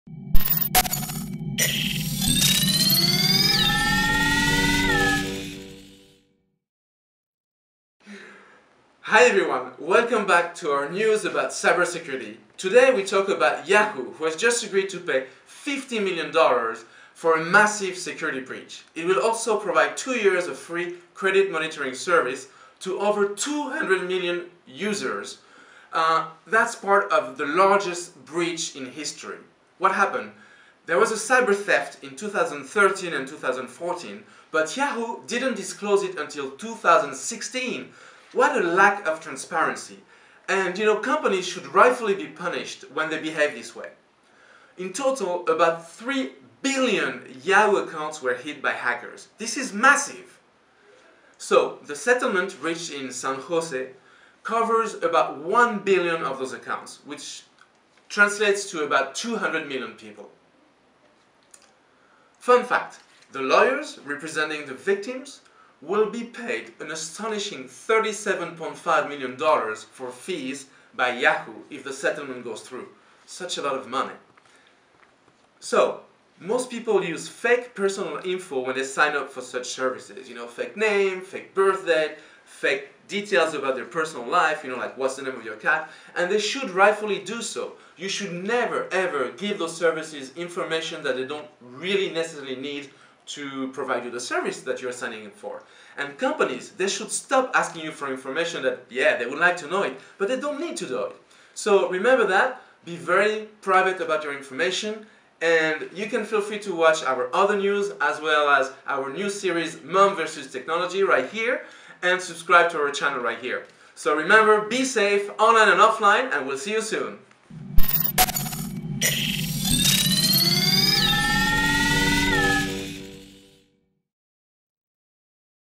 Yahoo has agreed to pay $50M in damage for massive security breach!
Frederic Thenault, CEO of Syneidis, presents another brief episode of cybersecurity news.

It will also provide two years of free credit-monitoring services to 200 million people whose email addresses and other personal information were stolen as part of the largest security breach in history.
Remember to be safe online and offline, and see you soon.